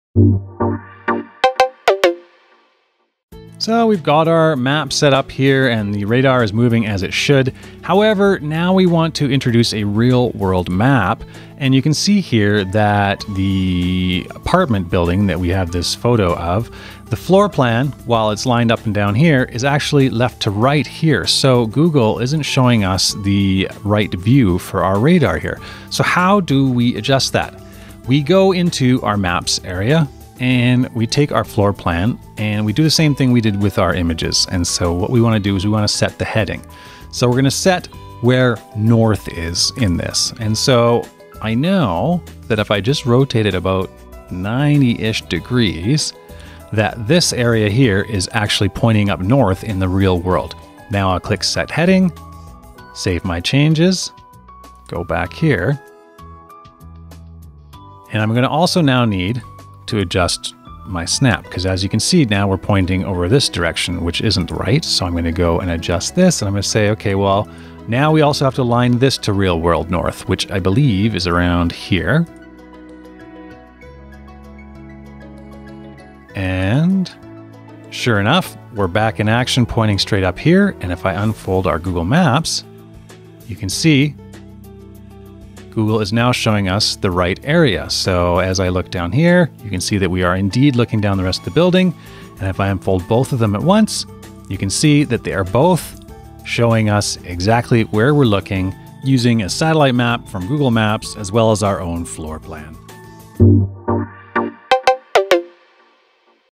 Aligning your Floor Plans with Google Maps in your SeekBeak 360º Virtual Tour
today!
In this video we take a look at using the Heading feature, using both the built in Maps/Floor Plans in conjunction with Google Maps.